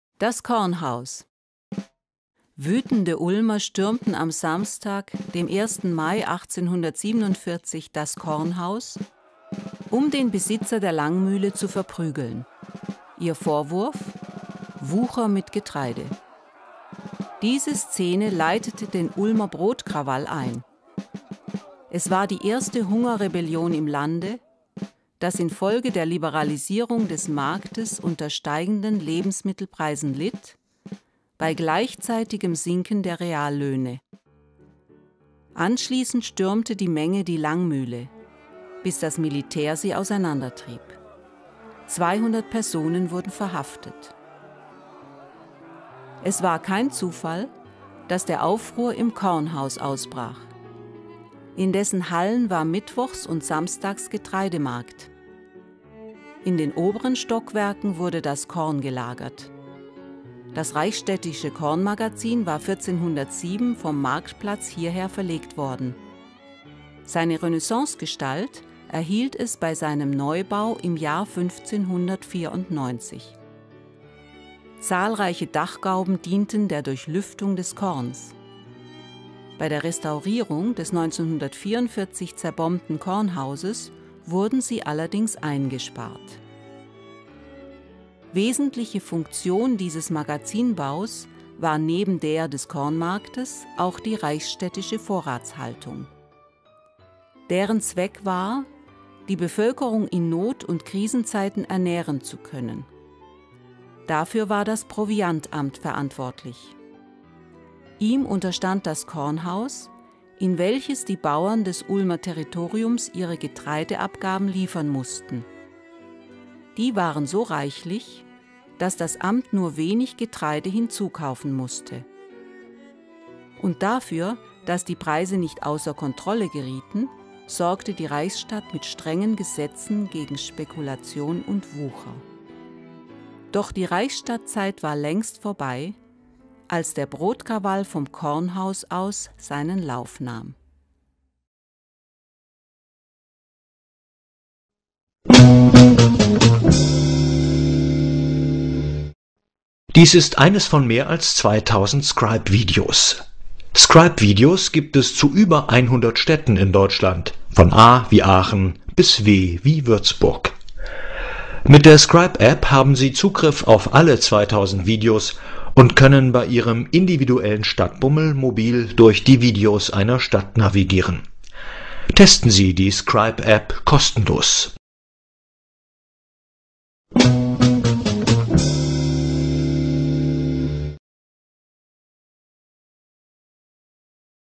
Ulmer Kornhaus - wütende Bürger stürmen die Speisekammer der Stadt Ulm.
Wütende Ulmer stürmten am Samstag, dem 1. Mai 1847 das Kornhaus, um den Besitzer der Langmühle zu verprügeln. Ihr Vorwurf: Wucher mit Getreide. Diese Szene leitete den „Ulmer Brotkrawall“ ein. Es war die erste Hungerrebellion im Lande, das infolge der Liberalisierung des Marktes unter steigenden Lebensmittelpreisen litt – bei gleichzeitigem Sinken der Reallöhne. Anschließend stürmte die Menge die Langmühle, bis das Militär sie auseinandertrieb. 200 Personen wurden verhaftet.
Es war kein Zufall, dass der Aufruhr im Kornhaus ausbrach. In dessen Hallen war mittwochs und samstags Getreidemarkt. In den oberen Stockwerken wurde das Korn gelagert. Das reichsstädtische Kornmagazin war 1407 vom Marktplatz hierher verlegt worden. Seine Renaissance-Gestalt erhielt es bei seinem Neubau im Jahr 1594. Zahlreiche Dachgauben dienten der Durchlüftung des Korns. Bei der Restaurierung des 1944 zerbombten Kornhauses wurden sie allerdings eingespart.
Wesentliche Funktion dieses Magazinbaus war neben der des Kornmarktes auch die reichsstädtische Vorratshaltung. Deren Zweck war, die Bevölkerung in Not- und Krisenzeiten ernähren zu können. Dafür war das Proviantamt verantwortlich. Ihm unterstand das Kornhaus, in welches die Bauern des Ulmer Territoriums ihre Getreide-Abgaben liefern mussten. Die waren so reichlich, dass das Amt nur wenig Getreide hinzukaufen musste. Und dafür, dass die Preise nicht außer Kontrolle gerieten, sorgte die Reichsstadt mit strengen Gesetzen gegen Spekulation und Wucher.
Doch die Reichsstadtzeit war längst vorbei, als der Brotkrawall vom Kornhaus aus seinen Lauf nahm.